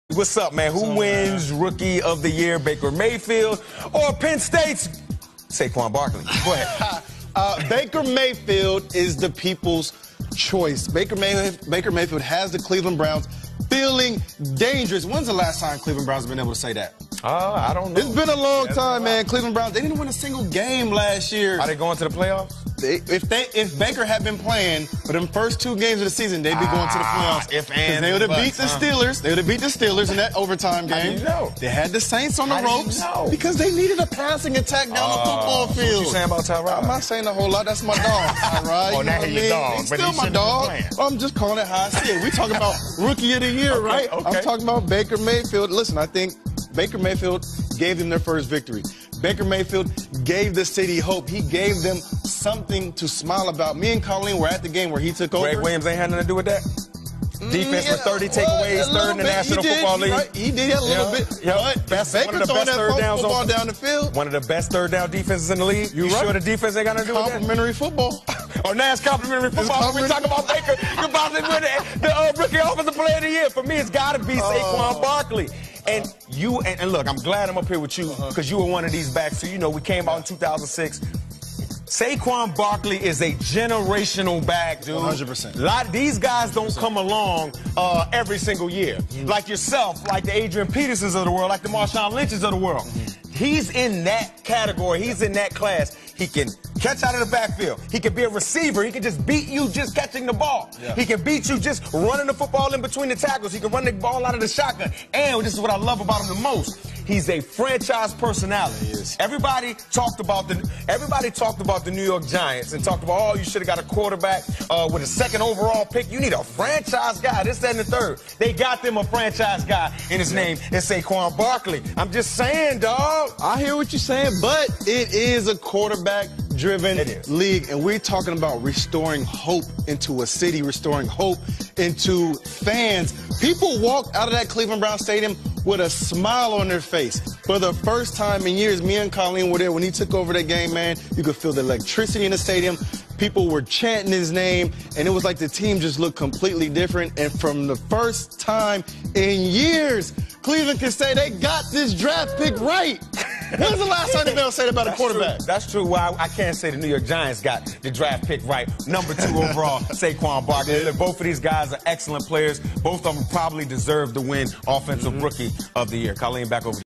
More deserving of Offensive Rookie of the Year: Mayfield or Barkley?
NFL Network's Michael Robinson and Reggie Bush discuss who is more deserving of Offensive Rookie of the year Baker Mayfield or Saquon Barkley?